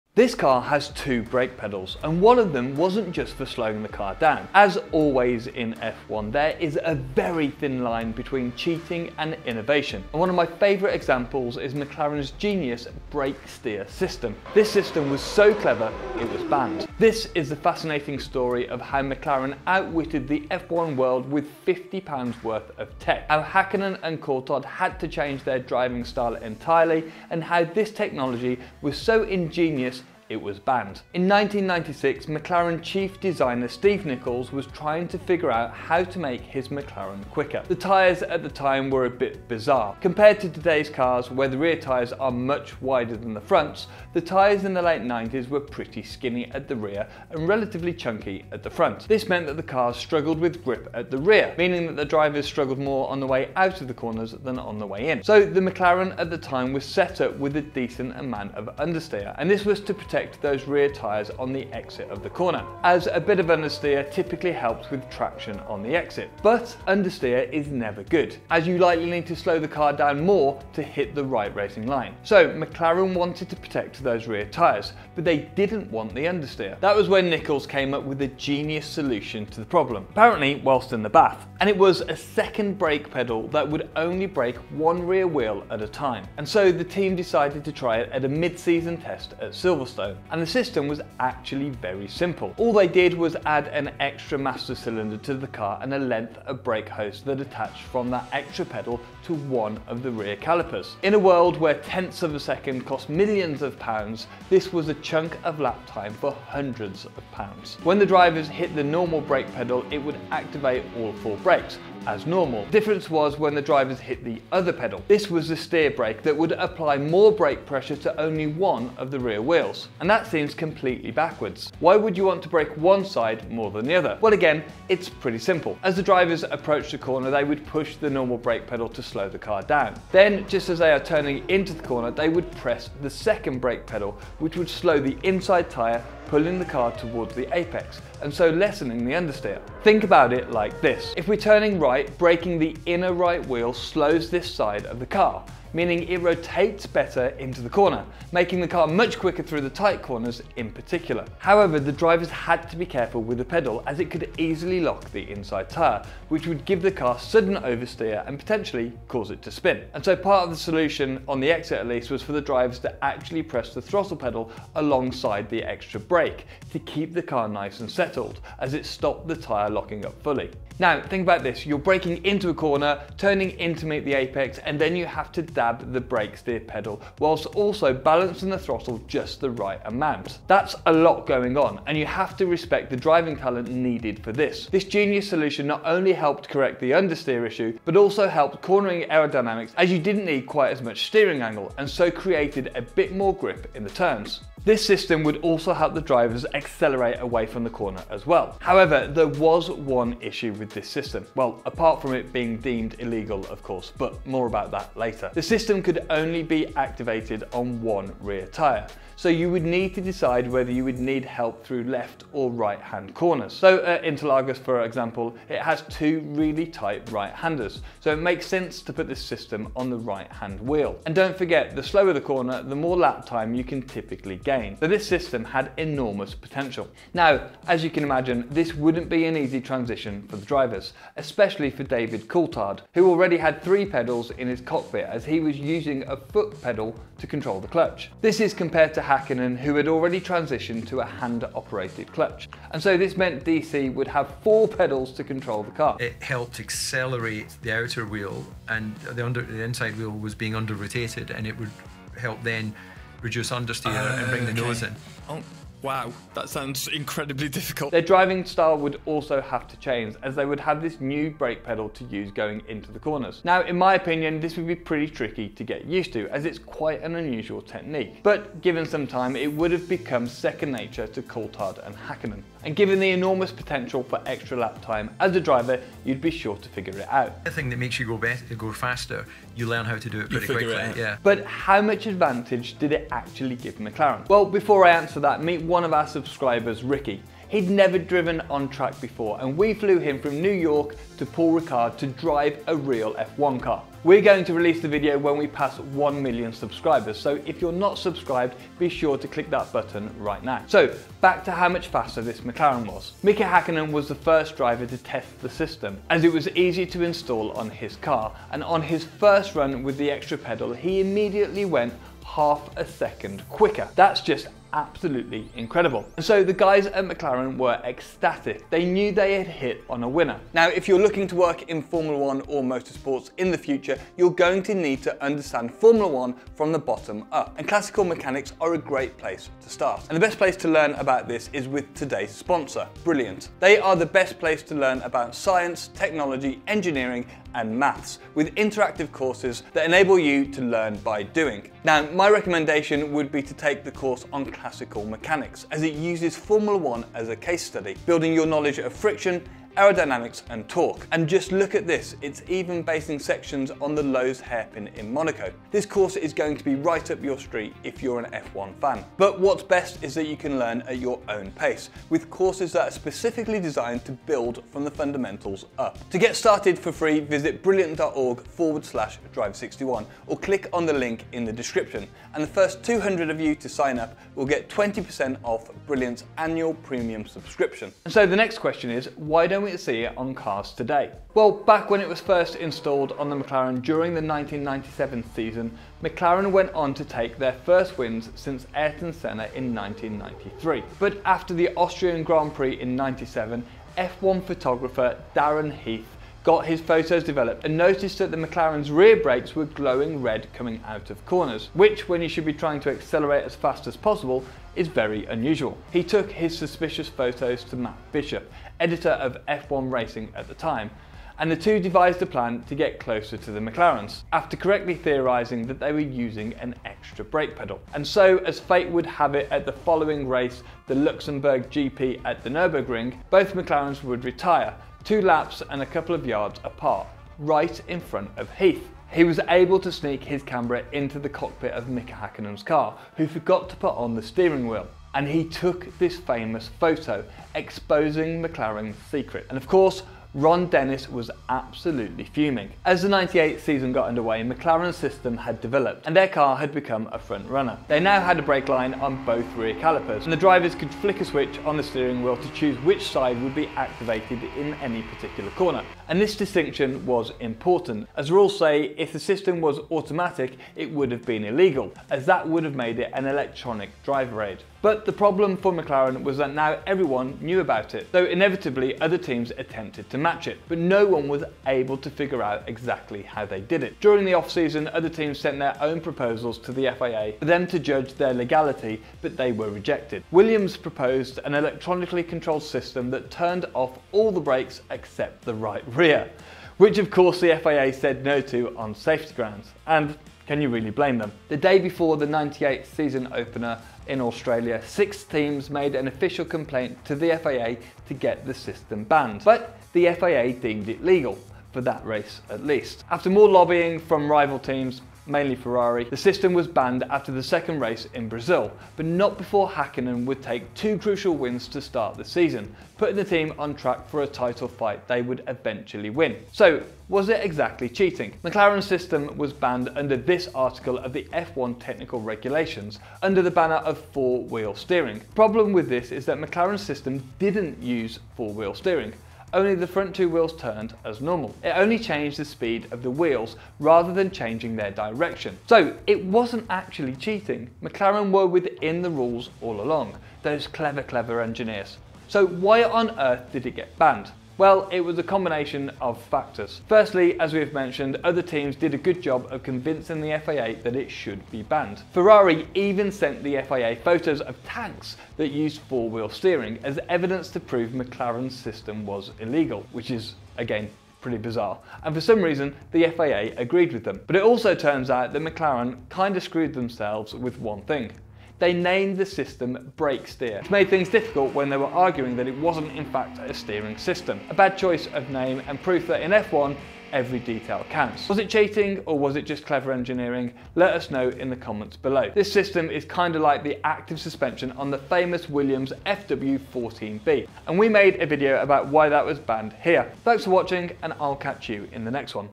Why This F1 Car Has FOUR PEDALS
Join our F1 Mailing List!

This F1 car has two brake pedals - and one of them didn't just slow the car down…

As always in F1, there is a VERY thin line between cheating and innovation. And one of my favourite examples is McLaren’s genius ‘Brake-Steer’ system.

The system was so clever it was banned.

This is the fascinating story of how McLaren outwitted the F1 world with £50 worth of tech , how Hakkinen and Coulthard had to change their driving entirely, and how this technology was so ingenious - it was banned.

In 1996, McLaren chief designer Steve Nicols was trying to figure out how to make his McLaren quicker.

The tyres at the time were a bit bizarre. Compared to today’s cars (where the rear tyres are MUCH wider than the fronts) - the tyres in the late 90s were pretty skinny at the rear and relatively chunky at the front.

This meant that the cars were very rear-limited - meaning you struggled more with corner exit than entry.

So the McLaren at the time was set up with a decent amount of understeer, and this was to protect those rear tyres on the exit from the corner. As an understeery setup typically helps with traction on exit.

But - understeer is never good. As you may need to over-slow the car to be hitting the correct racing line.

So - they wanted to protect those rear tyres, but they didn’t want the understeer.